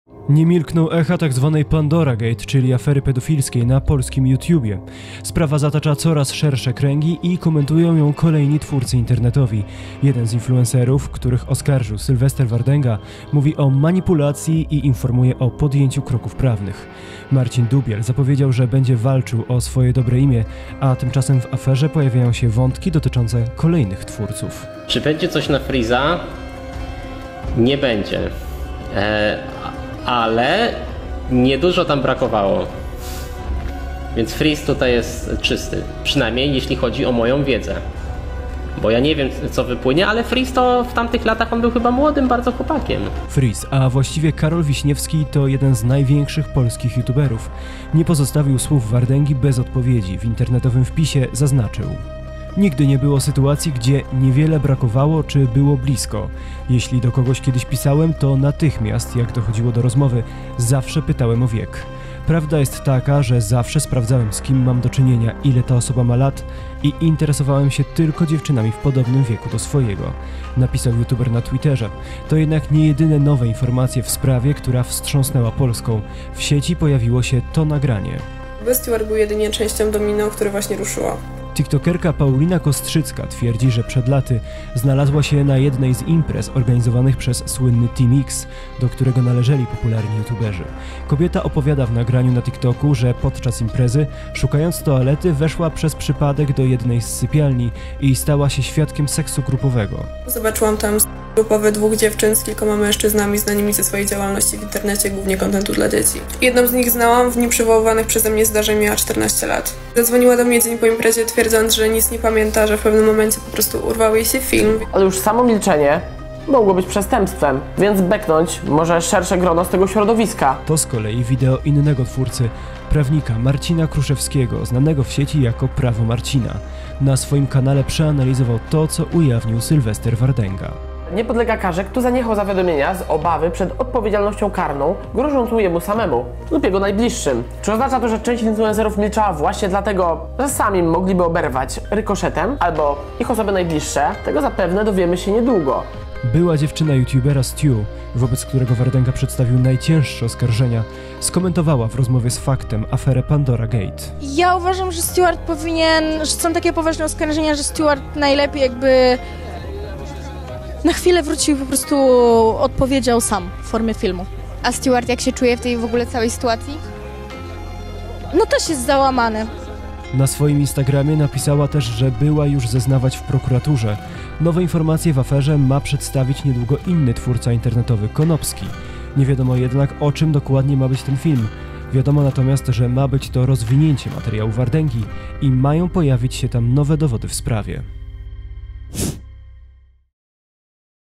Orgia w willi Teamu X? Kolejna influencerka zabrała głos! NOWE FAKTY w sprawie "Pandora Gate"
Skandal, który wybuchł wokół grupy polskich youtuberów, tylko wzbiera na sile. W sieci pojawił się głos kolejnej internetowej celebrytki, która twierdzi, że była przypadkowym świadkiem orgii z udziałem 14-letniej dziewczyny. Podkreśla, że nie był to odosobniony incydent, a takie obrzydliwe sytuacje zdarzały się częściej.